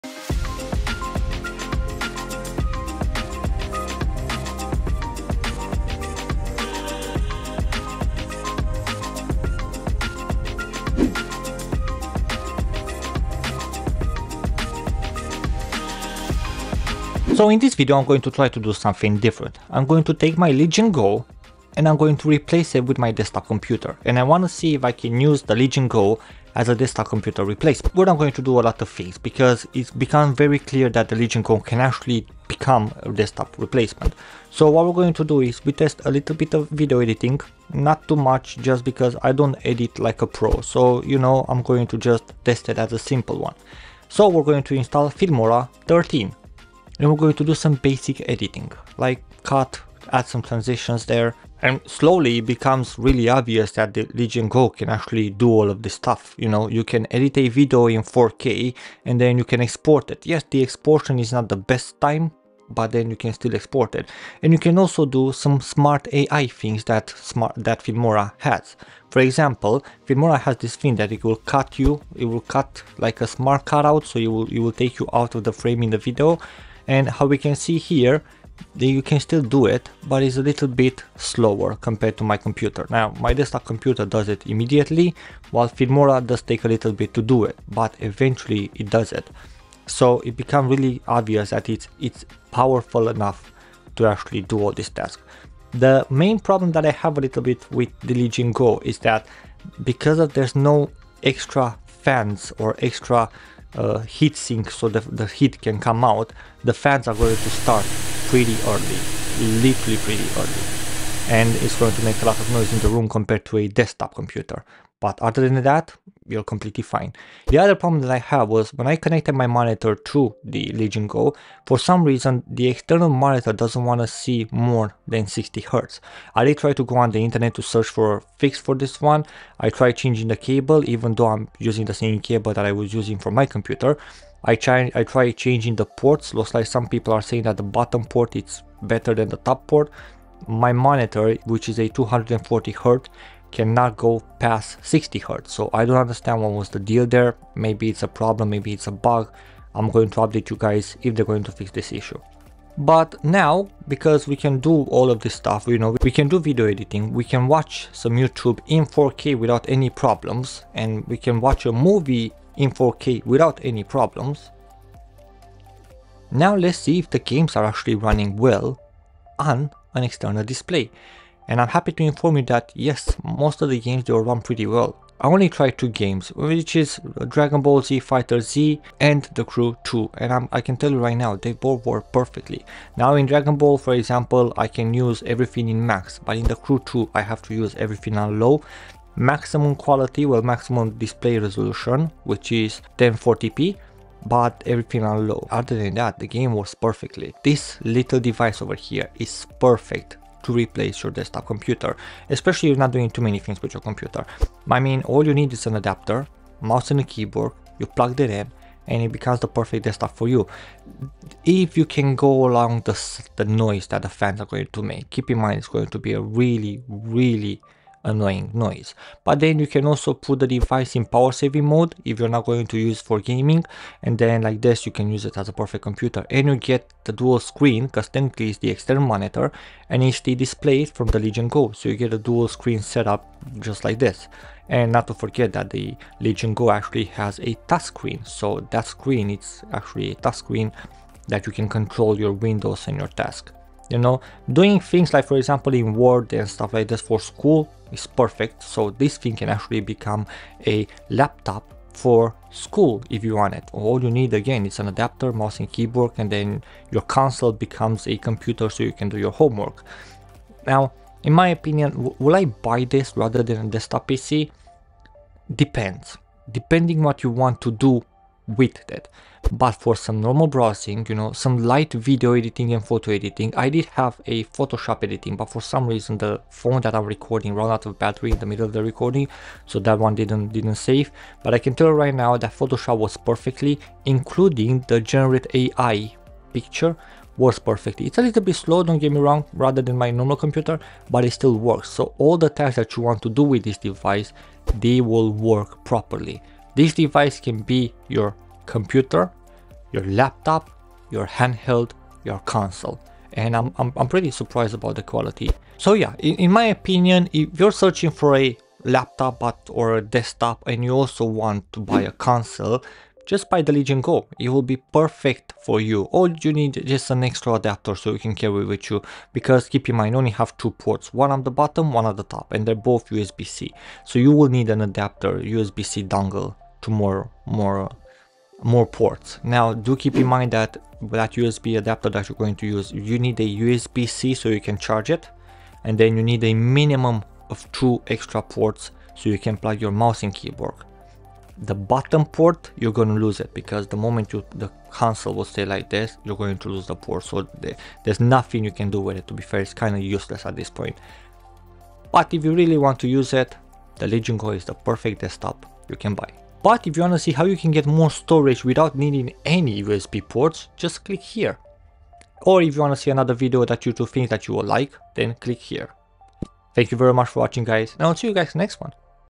Is the Lenovo Legion GO Powerful Enough to Ditch Your Desktop? Replacing my Destkop with Legion Go!
In this video i want to see if the legion go is powerful enough to replace a desktop computer. Due to the size difference you'll get a lot of benefits using it, but can it keep up with the tasks? As i understand we all have different needs from our computers, here i do the basic of Video Editing,Gaming,Browsing the internet,,Watching a 4K movie etc

For contact please scroll down.

Camera: Google Pixel 6 Pro
Microphone: Blue Yeti Nano